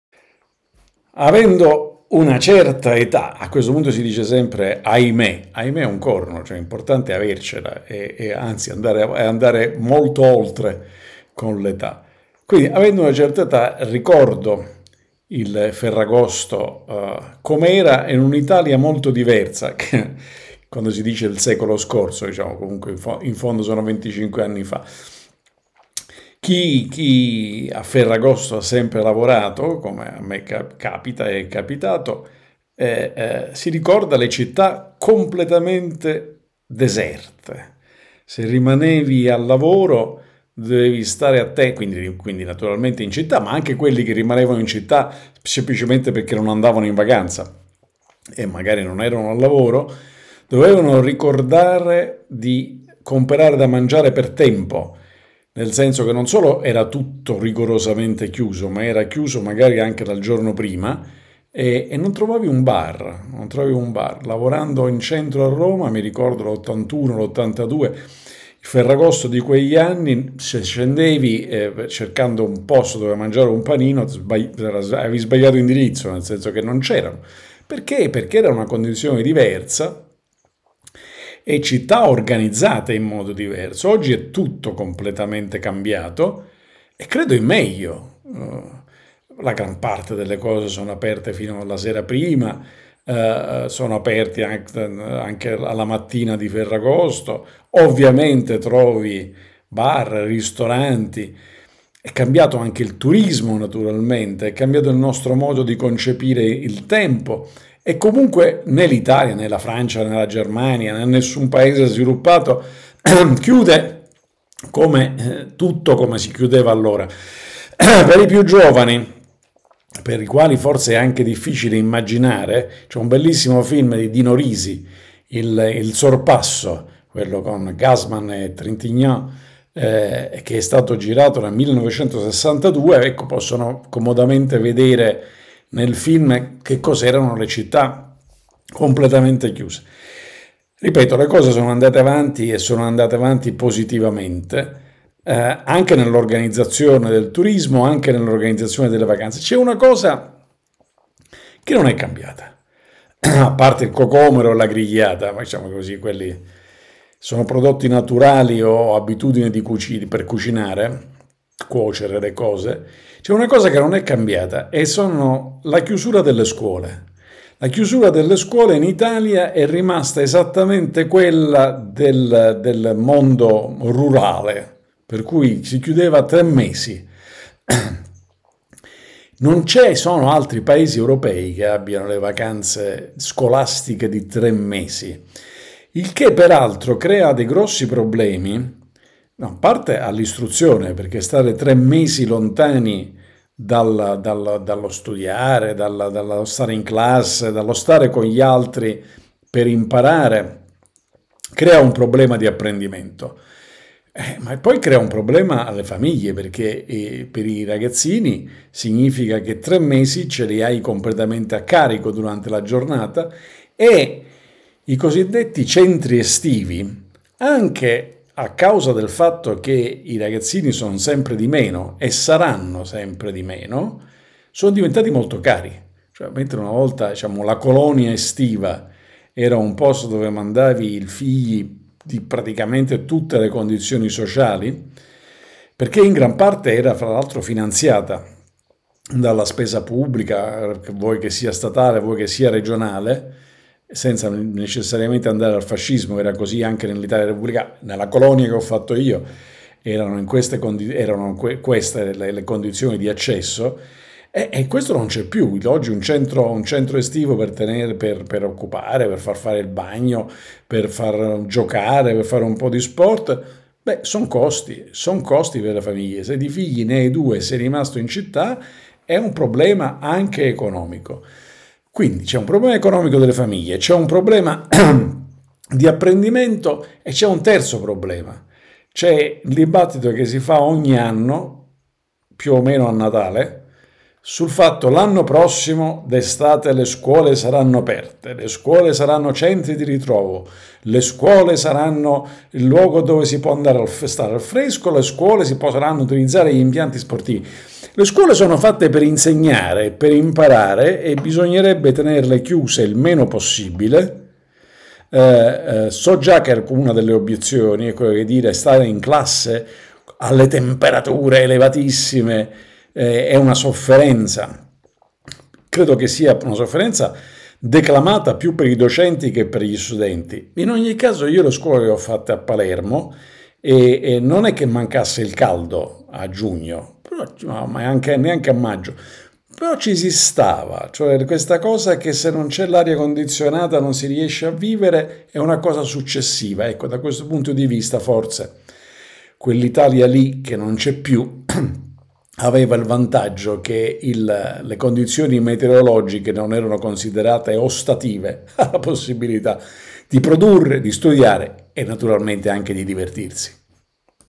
Ferragosto
Com'è cambiato il ferragosto degli italiani nel corso degli anni. Il commento del direttore Davide Giacalone.